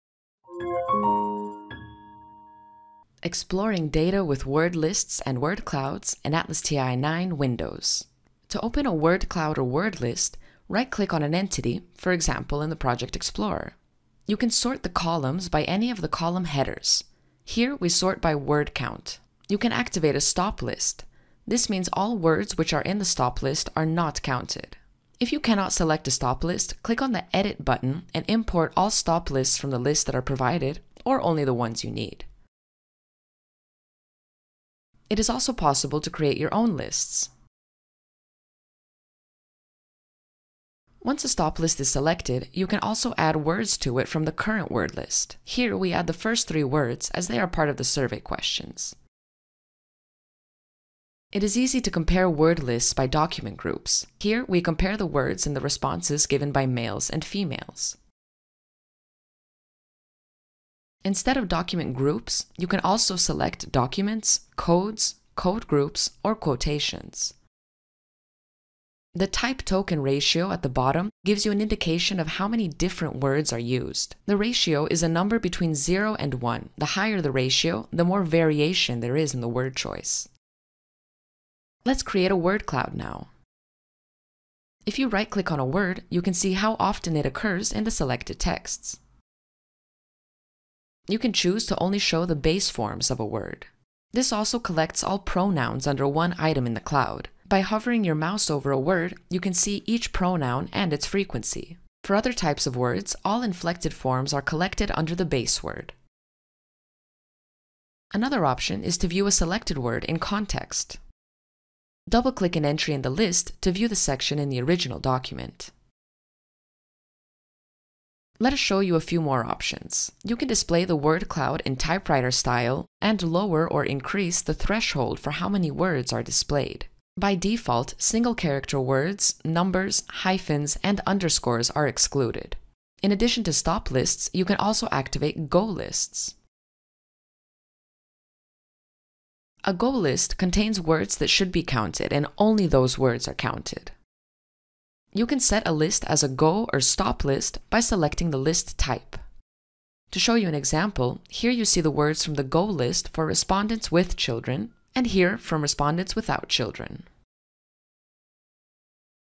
Word Lists and Word Clouds - ATLAS.ti Windows
A quick way to get a feeling for the content of text documents is by creating a word cloud or word list.